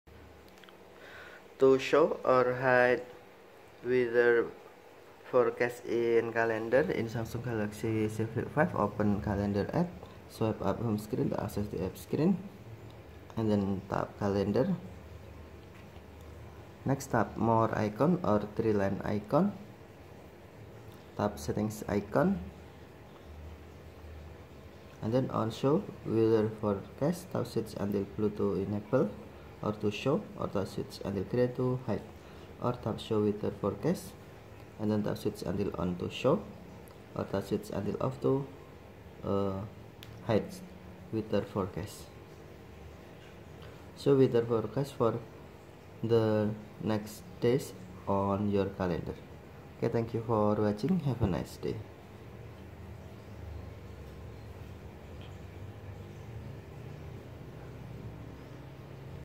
Samsung Galaxy Z Flip 5: How to Show or Hide Weather forecast in Calendar
The Samsung Galaxy Z Flip 5 provides users with a plethora of customization options to tailor their device to their preferences. Among these options is the ability to integrate weather forecasts directly into the calendar app. Whether you find it helpful to have weather information alongside your scheduled events or prefer a simpler calendar interface, adjusting this setting is quick and straightforward. In this guide, we'll walk you through the steps to show or hide weather forecasts in the calendar on your Galaxy Z Flip 5.

**Step 1: Open the Calendar App**

Begin by unlocking your Samsung Galaxy Z Flip 5 and locating the Calendar app on your home screen or in the app drawer. Tap on the Calendar app icon to open it.

**Step 2: Access Calendar Settings**

Once the Calendar app is open, navigate to the settings menu. This can usually be accessed by tapping on the three vertical dots or lines in the top-right corner of the screen, or by tapping on your profile picture or initials if you're using a Google Calendar account.

**Step 3: Find Weather Forecast Option**

In the Calendar settings menu, look for an option related to "Weather forecast" or "Show weather." This option may be located under the "General" or "View" section of the settings menu.

**Step 4: Toggle Weather Forecast On or Off**

Once you've located the Weather Forecast option, you'll typically see a toggle switch next to it. Simply tap on the toggle switch to turn the weather forecast on or off, depending on your preference.

**Step 5: Confirm Changes**

After toggling the Weather Forecast setting, exit the settings menu by tapping the back button or navigating away from the settings screen. Your changes will be automatically saved, and you'll now see weather forecasts displayed or hidden in the calendar view, according to your selection.

**Step 6: Verify Weather Forecast Display**

Return to the calendar view to confirm that weather forecasts are displayed or hidden as desired. If you've chosen to show weather forecasts, they should now appear alongside the dates in the calendar view. If you've opted to hide weather forecasts, they will no longer be visible.

By following these simple steps, you can easily customize the appearance of the calendar on your Samsung Galaxy Z Flip 5 to include or exclude weather forecasts based on your preferences.